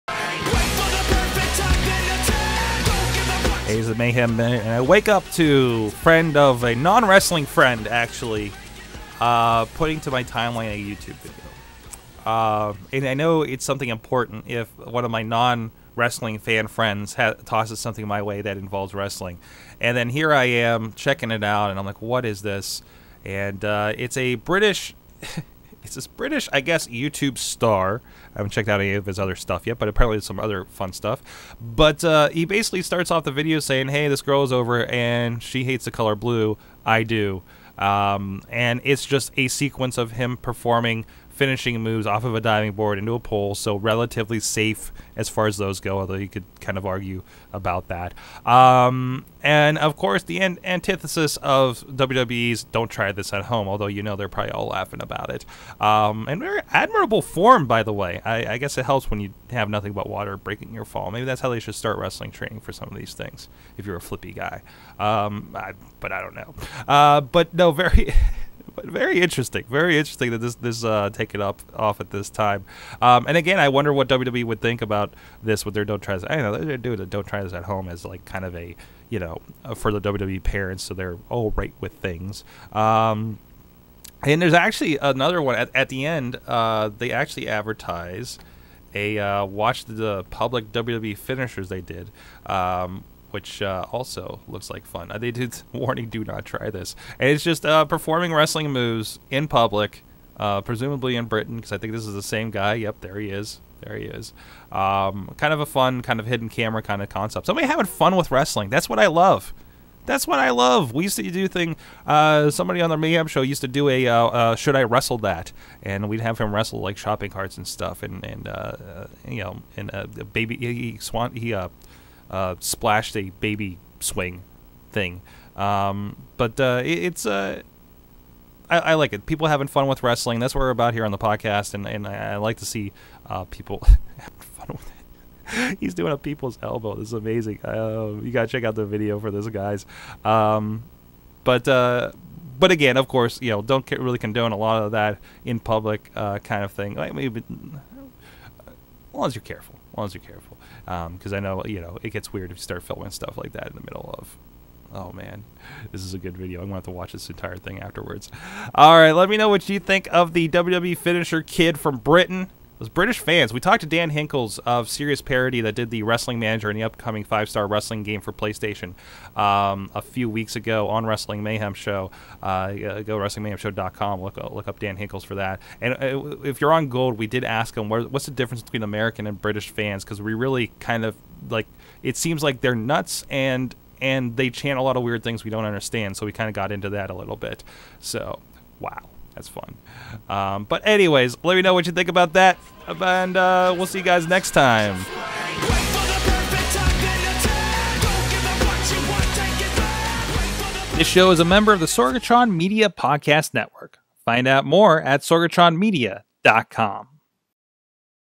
Mayhem Minute: Swimming Pool WWE Finishers
I awoke to find this british kid doing wrestling moves on a girl. So much for Don't Try This At Home...